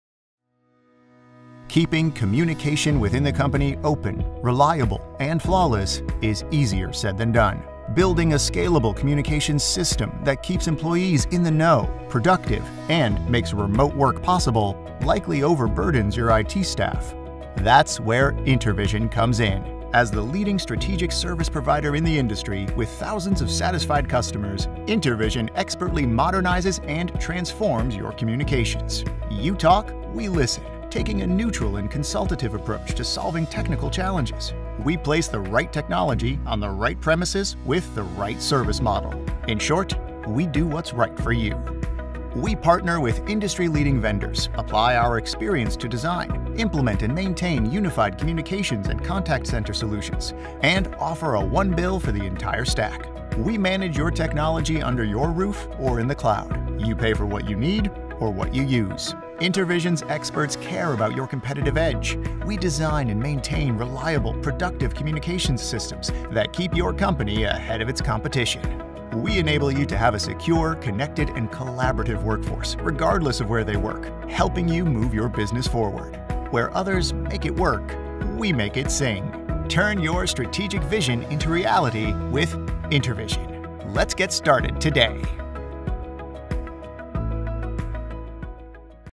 InterVision - At-a-Glance
We're InterVision, the leading managed service provider in the IT space. We deliver and support complex IT solutions for Mid-to-Enterprise & Public Sector clients in the US. We are uniquely positioned, based on both our deep On-Premises and Cloud expertise, to guide our Clients’ journeys. Our CX focus on client business outcomes help our Clients be more competitive, compliant and secure by leveraging our Partners’ innovative technology and our highly certified professionals.